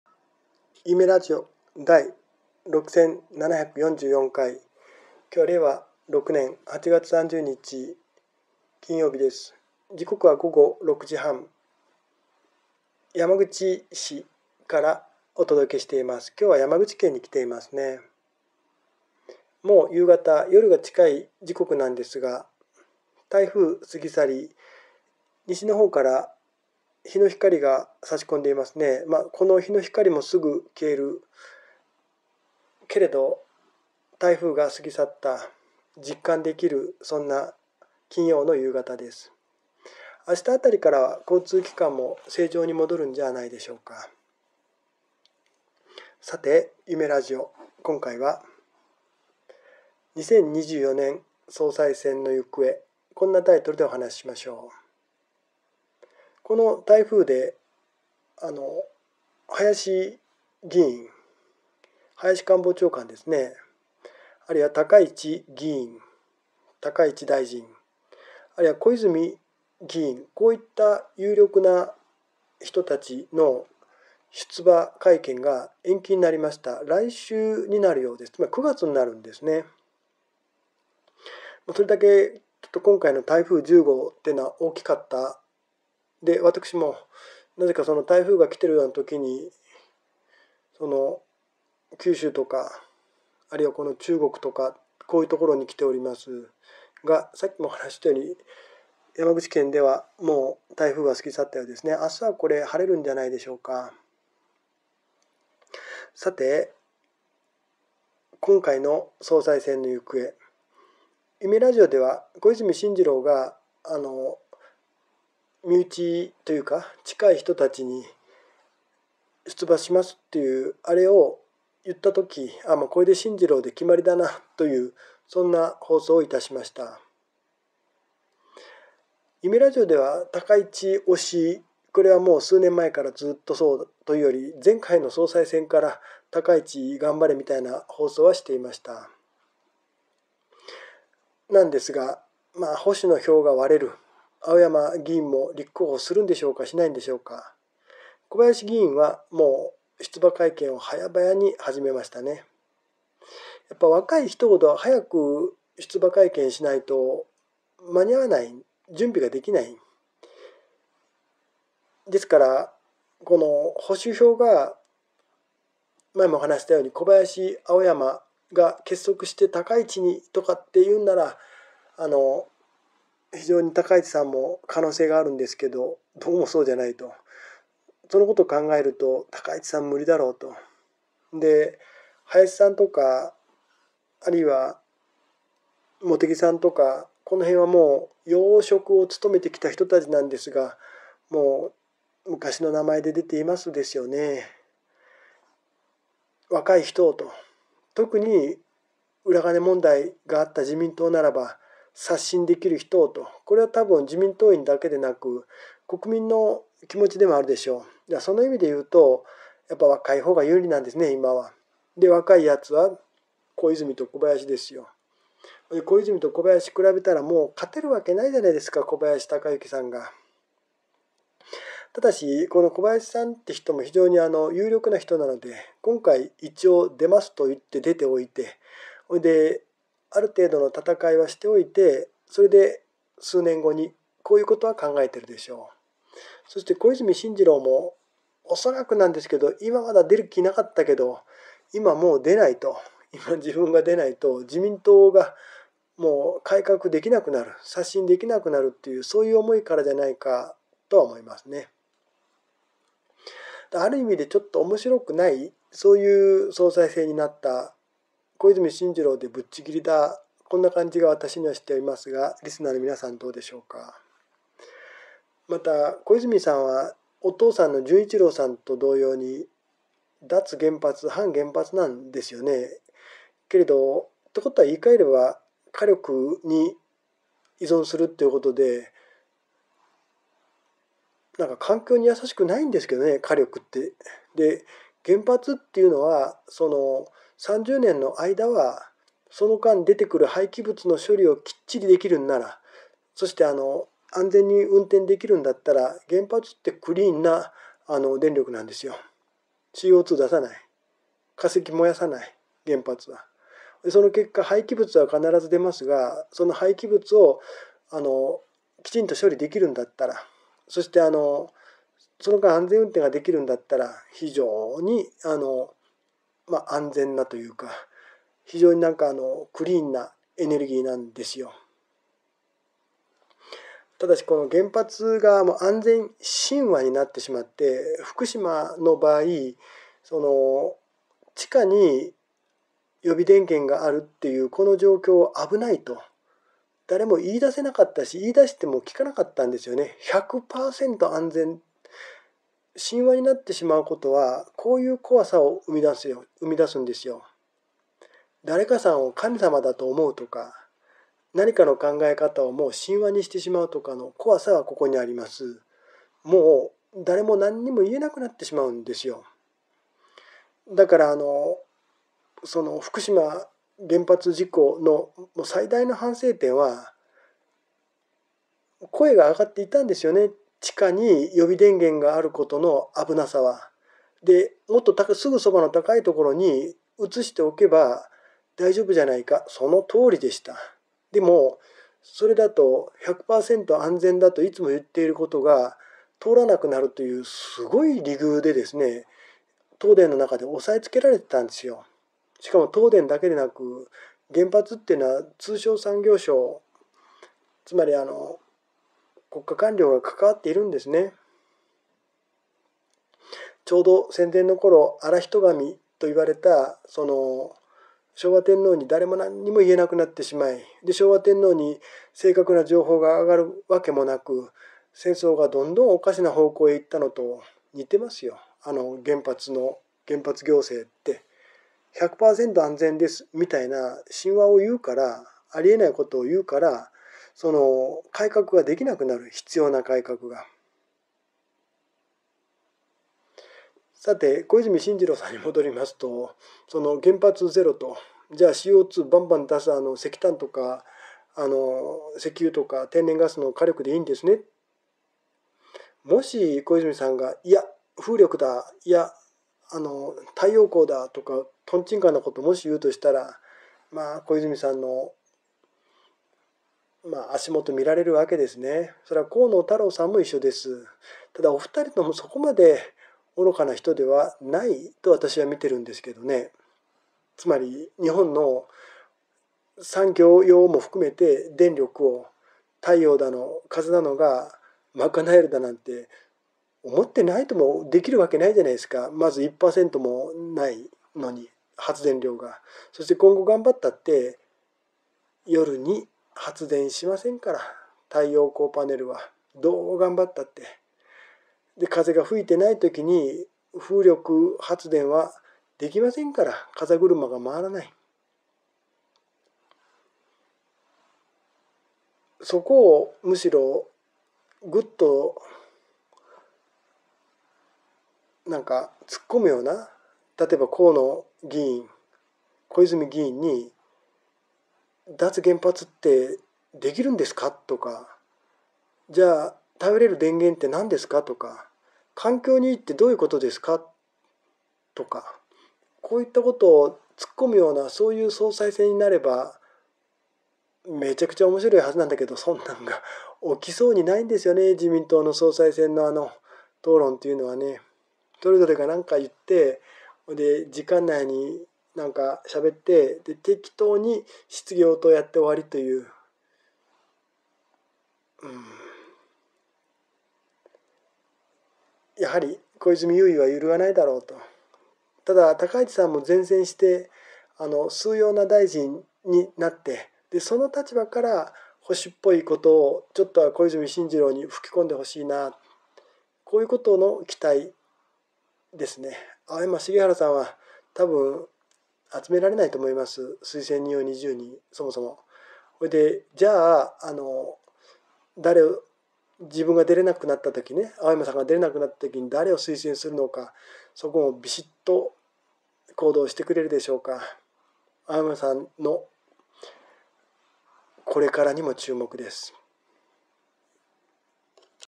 第6744回 2024年総裁選のゆくえ 2024.08.30
小泉進次郎有利